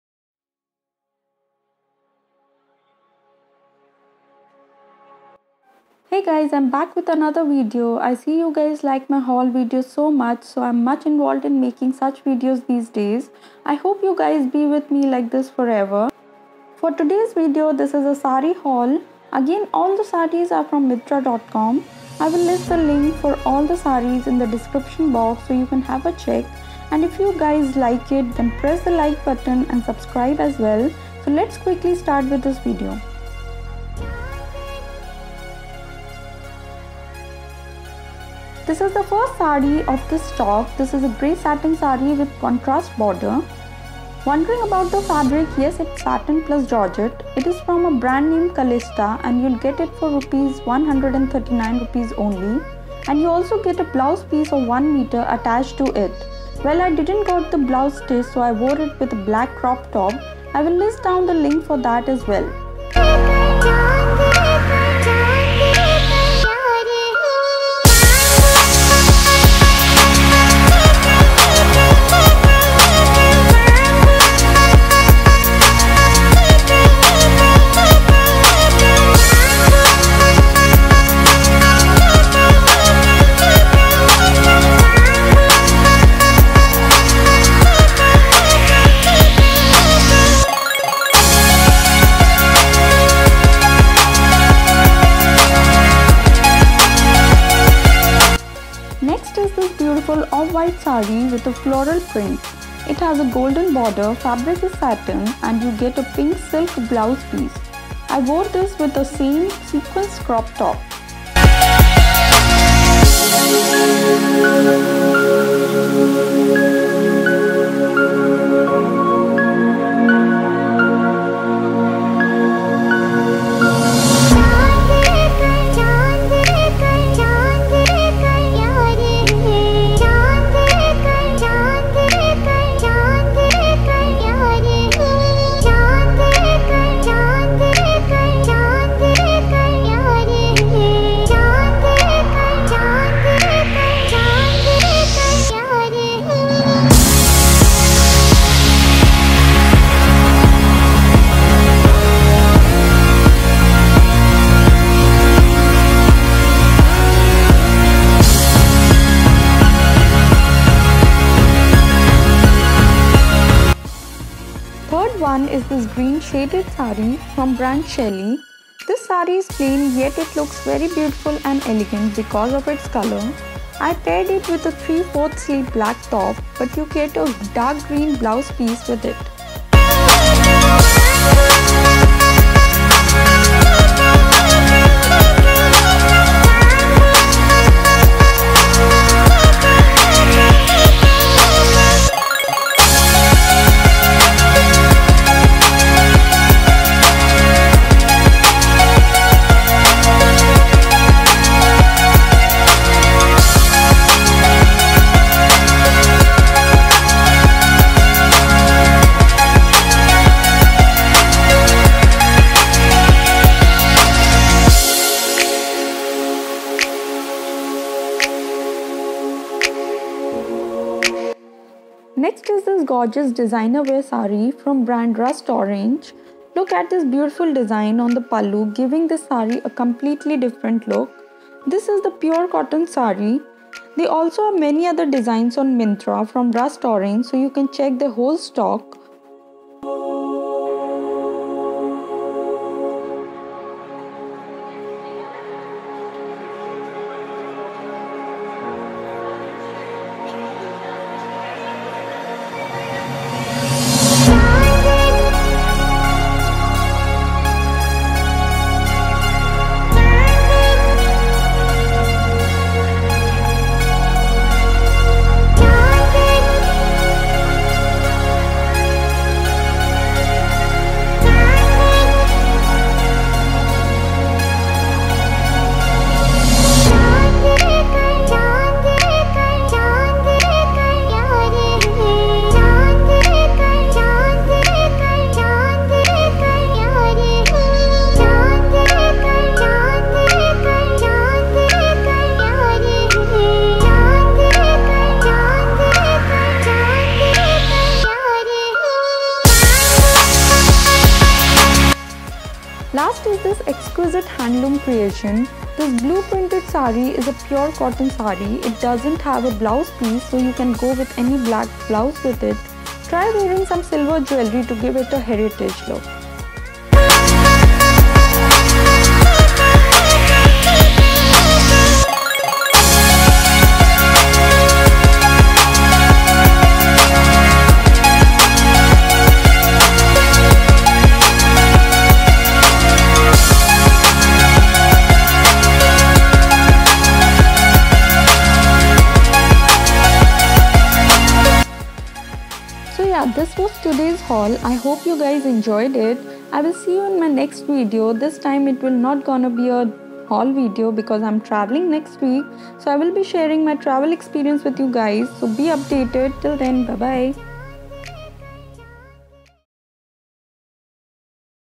SAREE HAUL || MYNTRA HAUL || 2018
heyyy ya guys !!!
its weekend..n m back with a saree haul...💄📿
ladies get ready to buy these beautiful saree. ❤

saree discription

so these were some styles..i hope u guys like them

thanks & love
adityaa😊